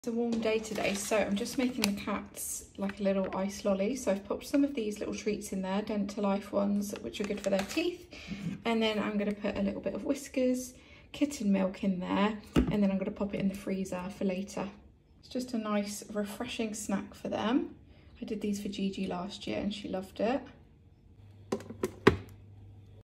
HOW TO KEEP CATS COOL IN THE SUMMER | CAT FRIENDLY ICE LOLLY
Here is a way I cool my cats down on a hot day. Follow @lizaprideaux for more videos about my British shorthair cats.